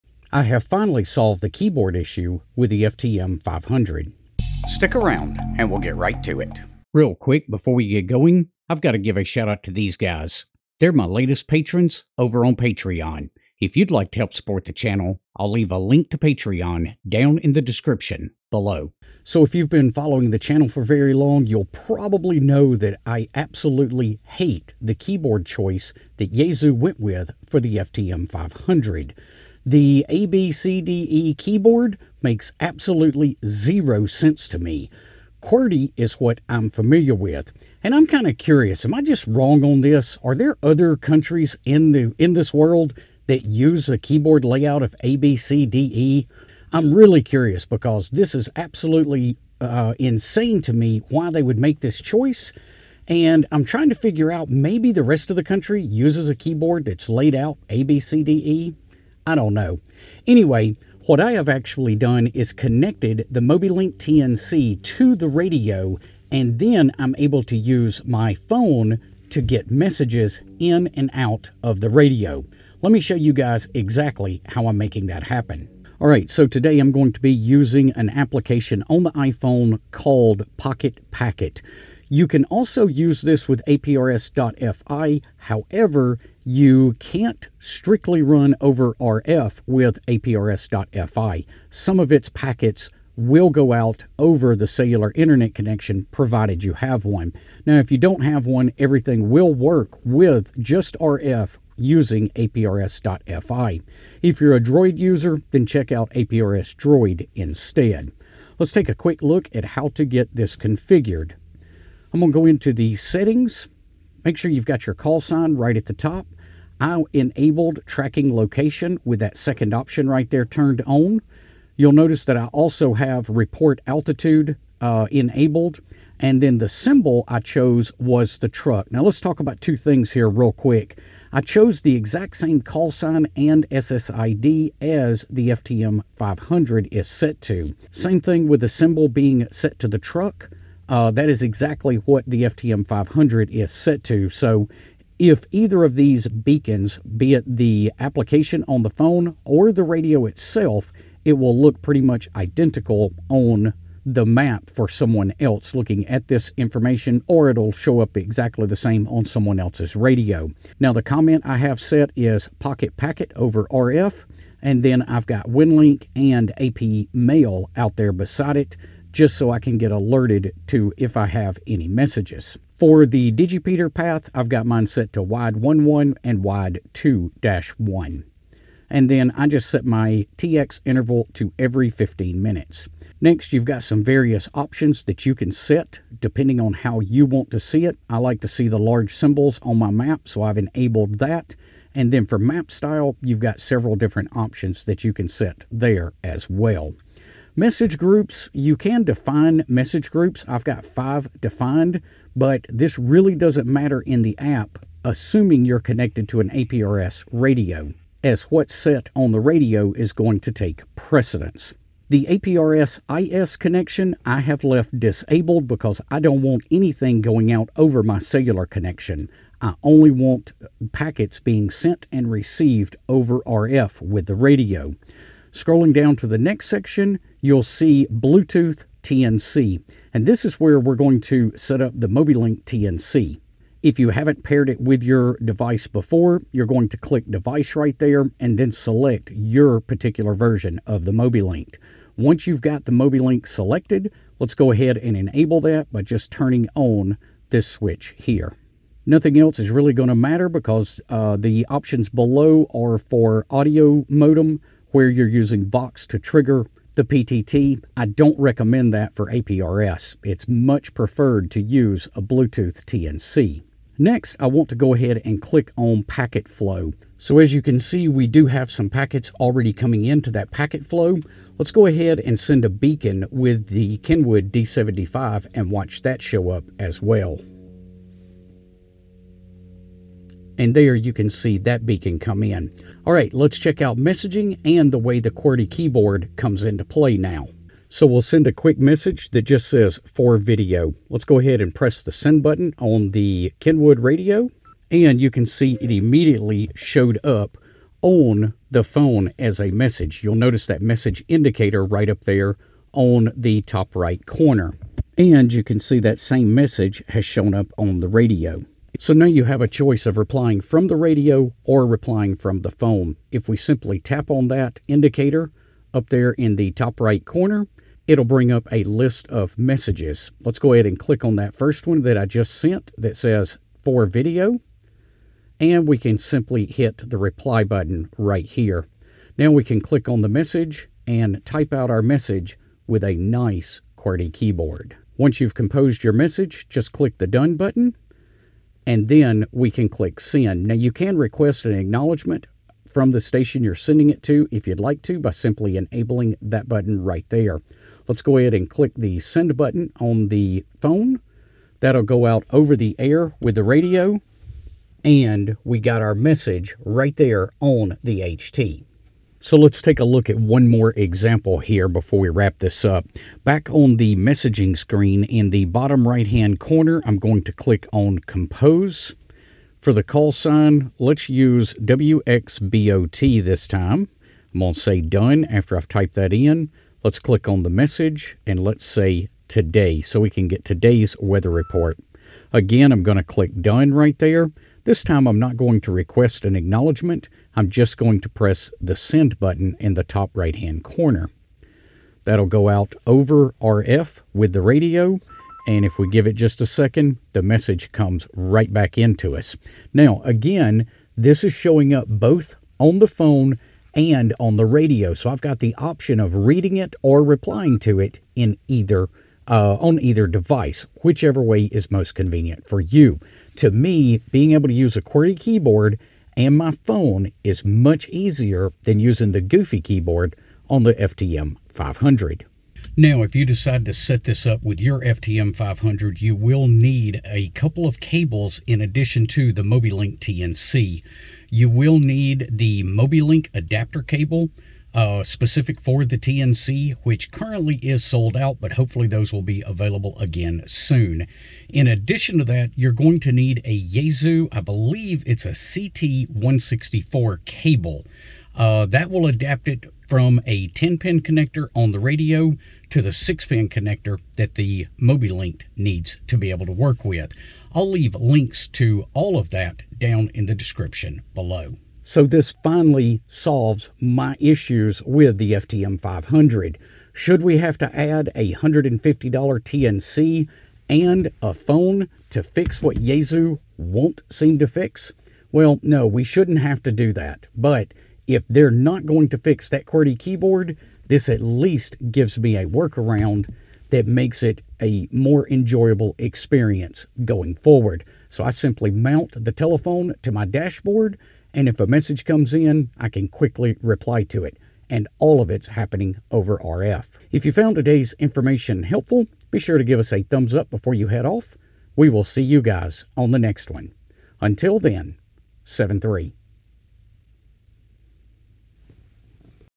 QWERTY Keyboard with Yaesu FTM500
The FTM500 is a great radio for voice. The user interface leaves a LOT to be desired in my opinion. We can utilize Pocket Packet on our phone to make the interface of the FTM500 much better with a QWERTY keyboard.